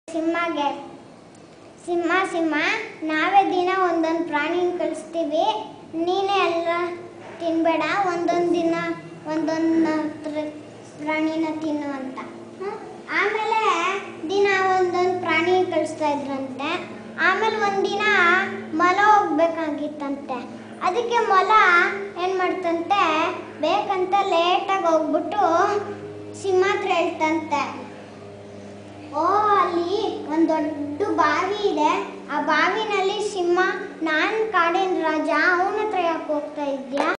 The Rabbit and the lion story by Annet Ashish Jagirdar at Mathru Blind School Yelahanka
The children of Mathru blind school in Yelahanka listening to Annet Ashish Jagirdar son of Rtn Kishor Jagirdar narrating a story of the lion and the rabbit in kannada.Rotary Learning centre  a part of Rotary Club of Yelahanka is associated with this school on project "Higgina Buggi" (Spring of Joy) Project Co-ordinator Kishor Jagirdar